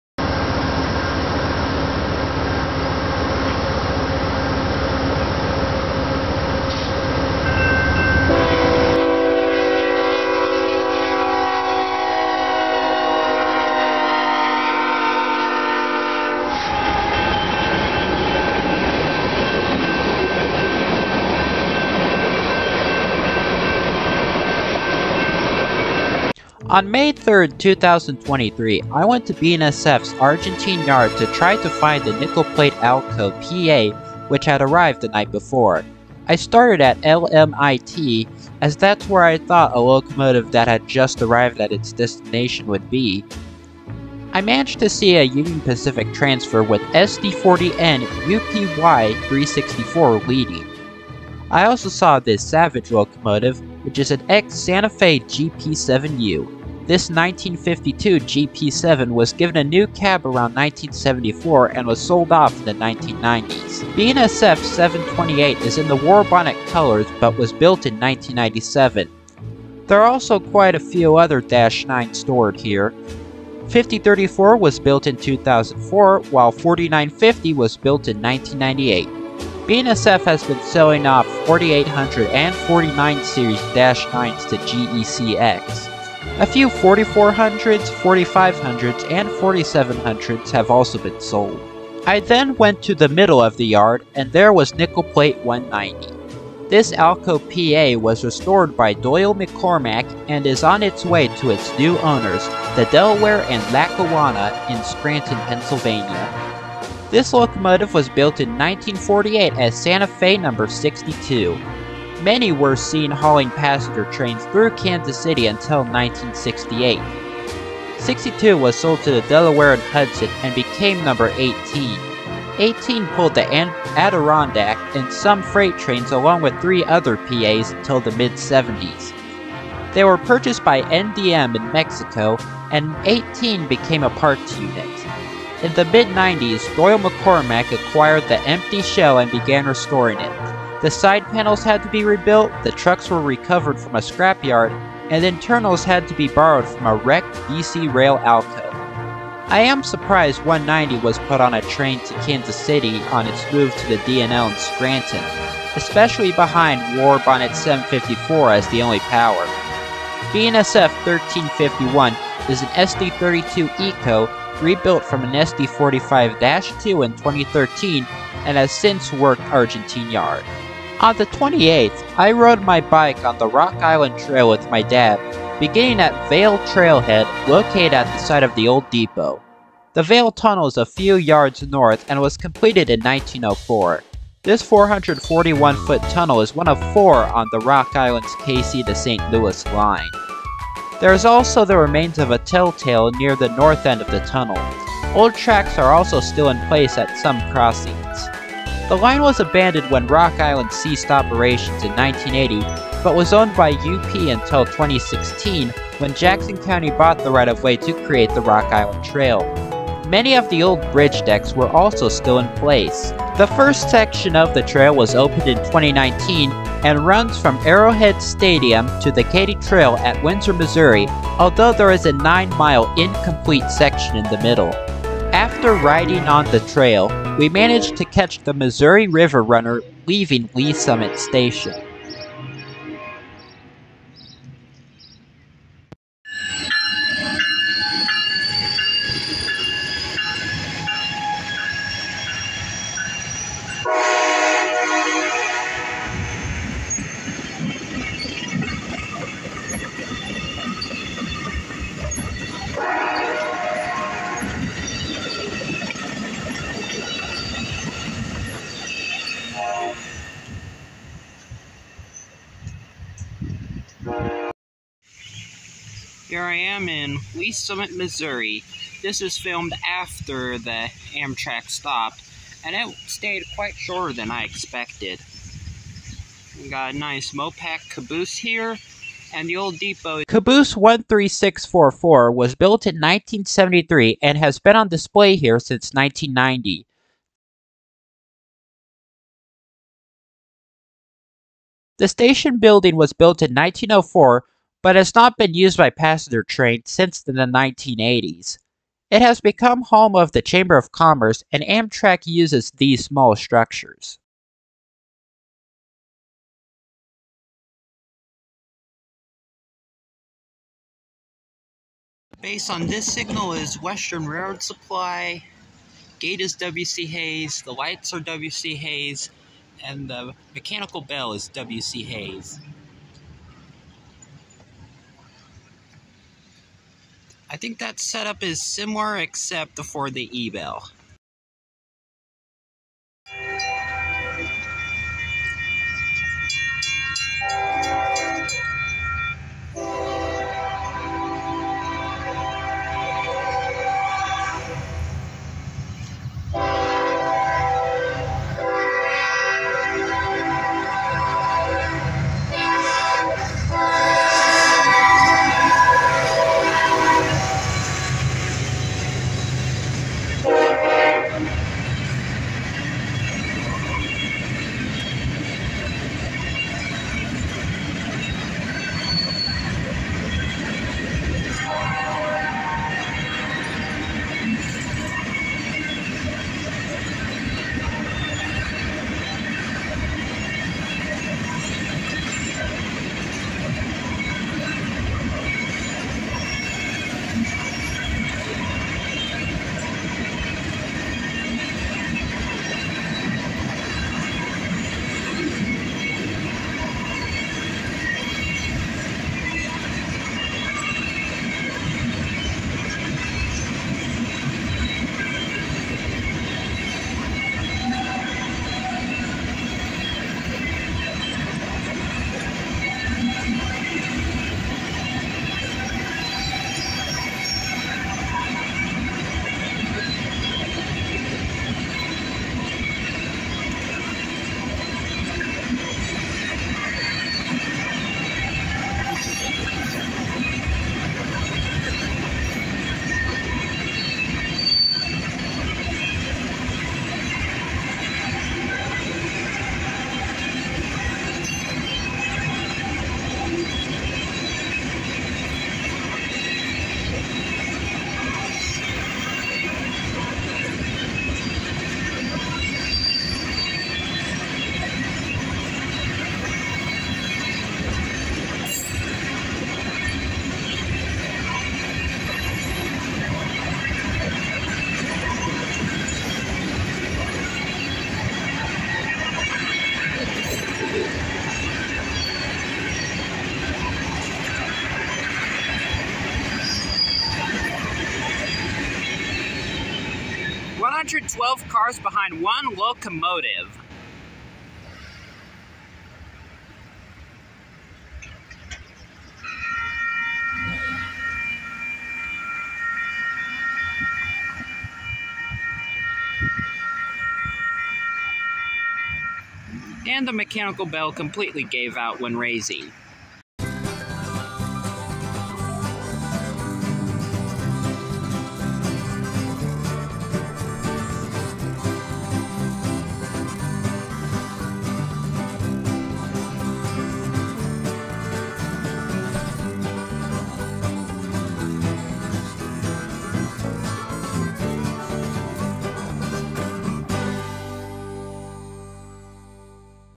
Trains Around Kansas City May 2023
ALCO PA Nickel Plate 190 is seen in Argentine Yard on its way to its new home in Scranton. Other unique power is also seen in Argentine Yard.
I also went on the Rock Island Trail and saw the Missouri River Runner later that same month, which I combined with this video.
All of the pictures and videos I took on these two visits were done on my phone, which wasn't as good as with a proper camera.